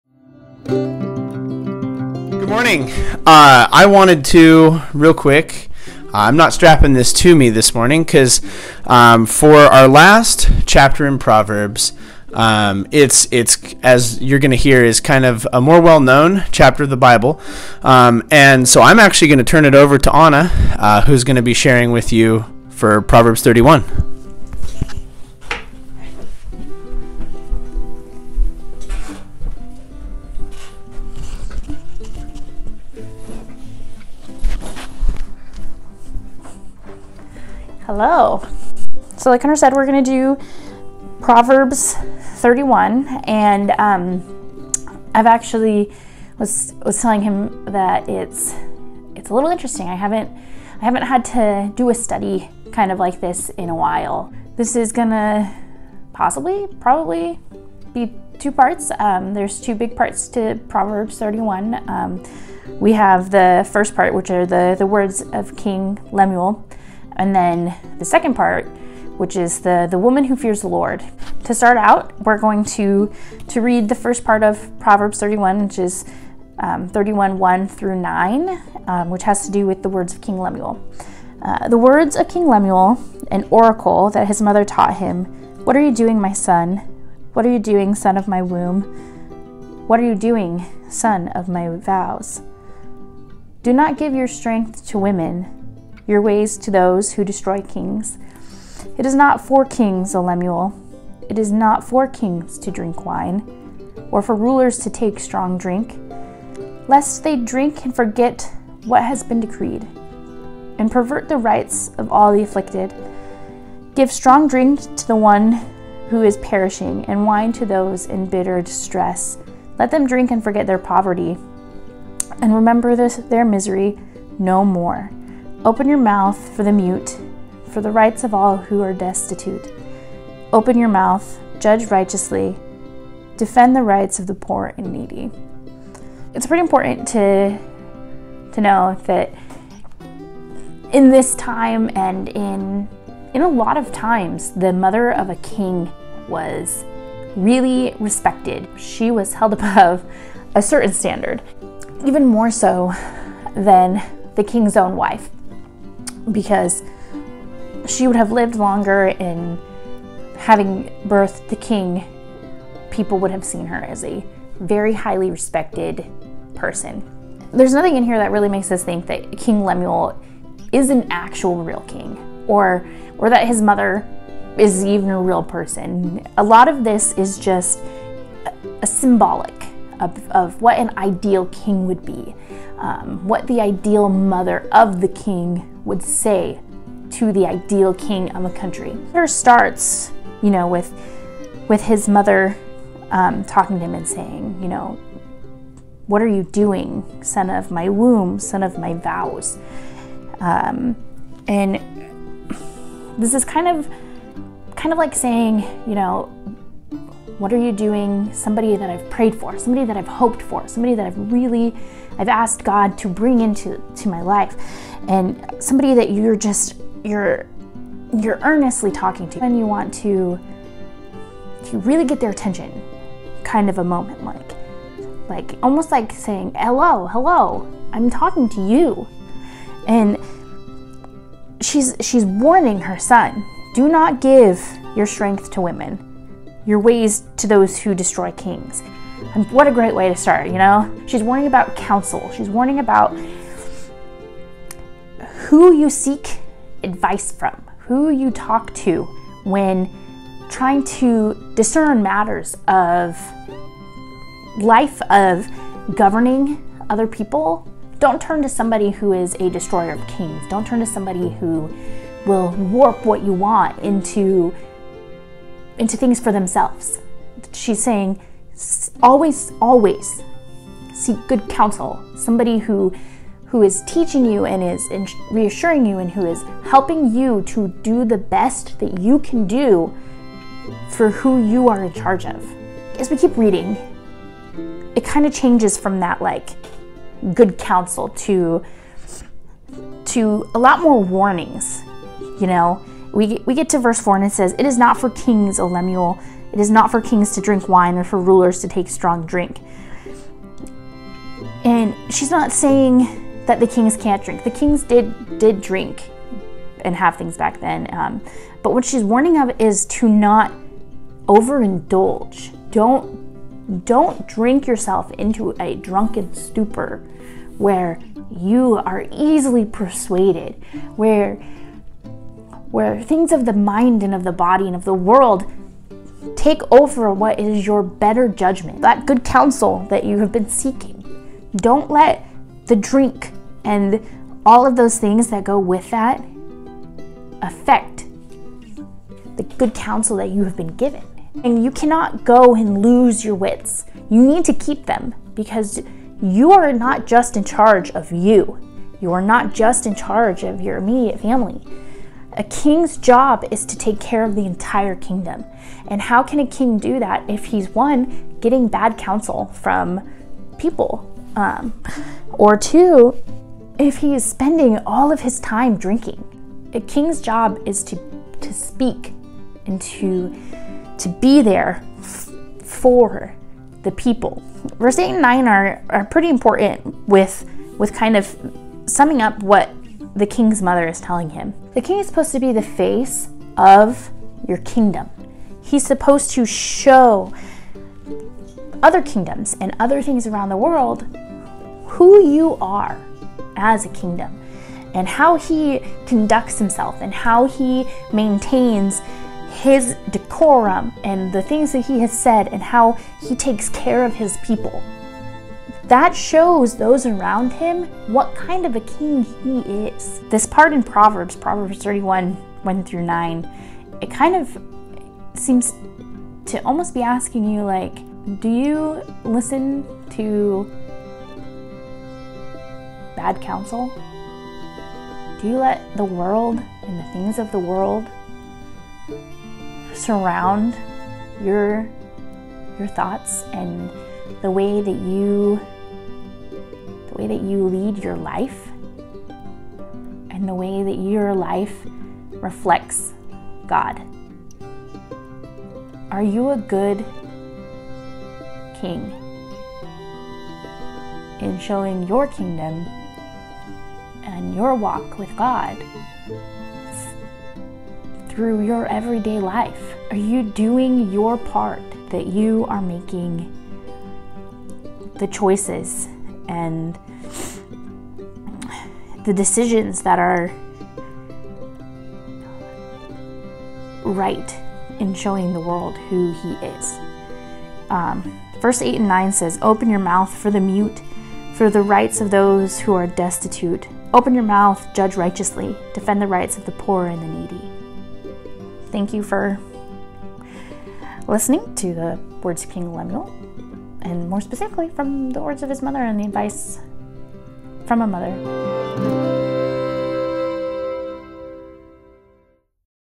Proverbs Part 108: A Mother Knows Best
Proverbs 31:1-9

Ana shares about the advice King Lemuel’s mother gave him regarding being a good king.